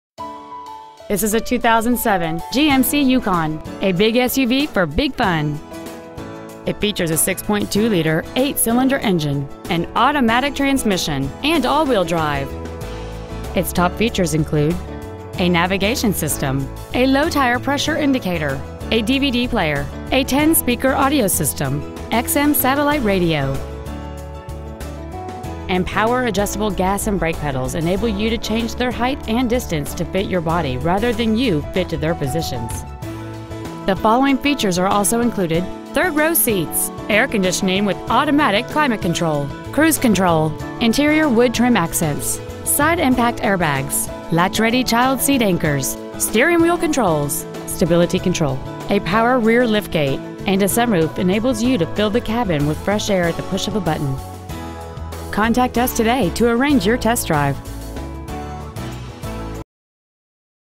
2007 GMC Yukon Denali Dallas TX
We are proud to present this 2007 GMC Yukon Denali .

 Year : 2007
 Make : GMC
 Model : Yukon Denali
 Engine : 6.2L Vortec 1000 VVT V8 SFI Engine
 Trans . : Automatic
 Exterior : White
 Miles : 181,508
 Interior : Tan

 Pre-Owned 2007 GMC Yukon Denali Arlington TX